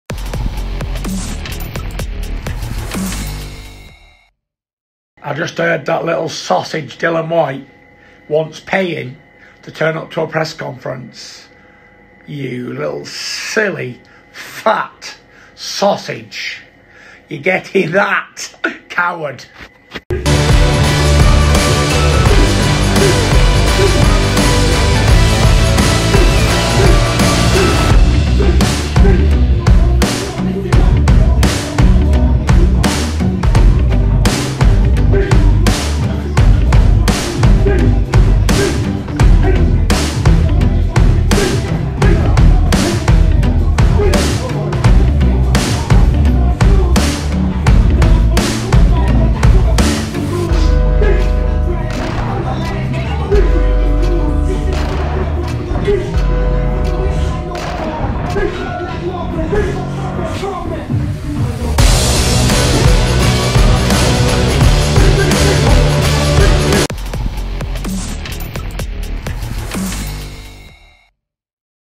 "I'VE HEARD DILLIAN WHYTE WANTS PAYING TO TURN UP TO A PRESS CONFERENCE... COWARD!" | TYSON FURY
WBC and Lineal World Heavyweight Champion Tyson Fury blasts Dillian Whyte for allegedly wanting more money to turn up to a press conference.